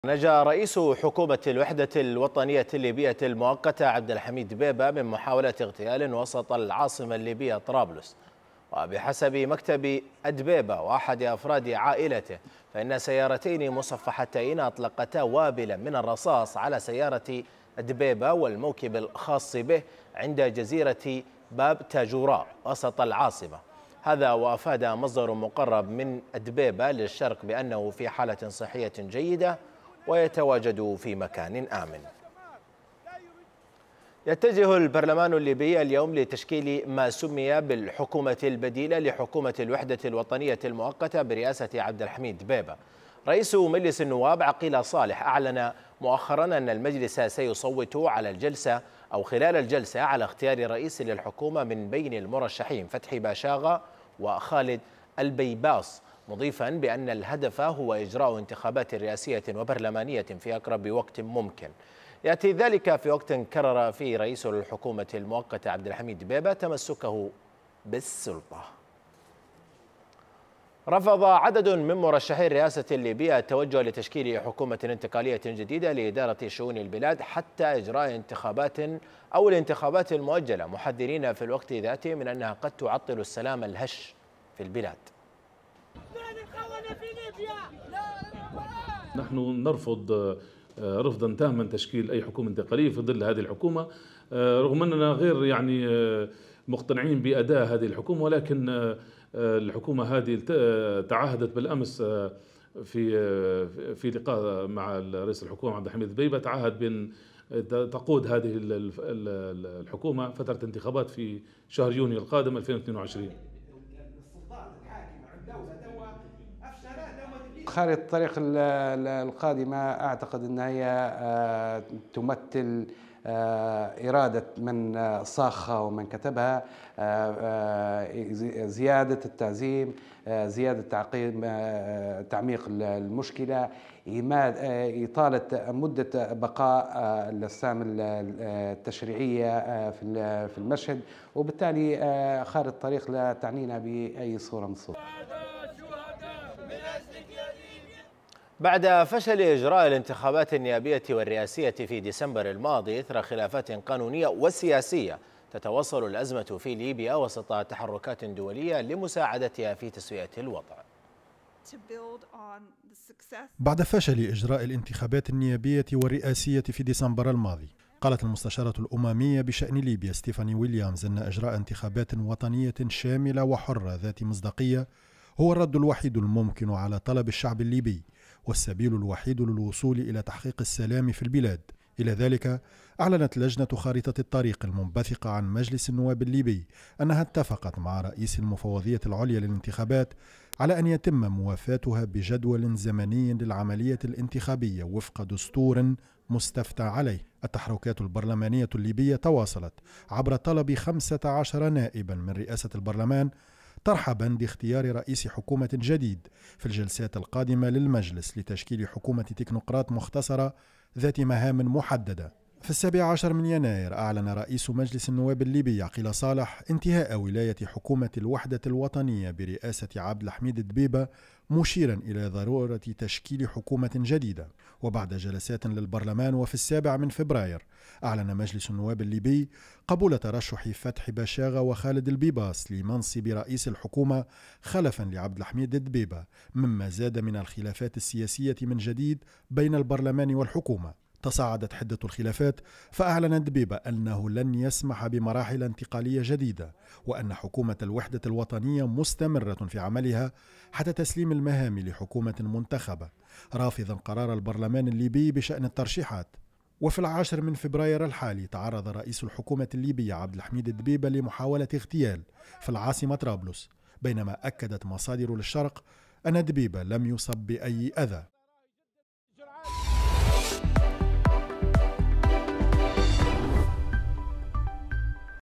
نجاة رئيس الوزراء الليبي من محاولة اغتيال في طرابلس - أخبار الشرق
قالت مصادر مقربة من رئيس الحكومة الليبية عبد الحميد الدبيبة، إنه نجا دون أذى من محاولة اغتيال عندما أطلق مجهولون النار على سيارته بطرابلس في الساعات الأولى من صباح الخميس وأضافت المصادر لـ"الشرق"، إن الدبيبة تعرض لمحاولة الاغتيال عند جزيرة باب تاجوراء وسط العاصمة الليبية، أثناء توجهه نحو منطقة النوفليين.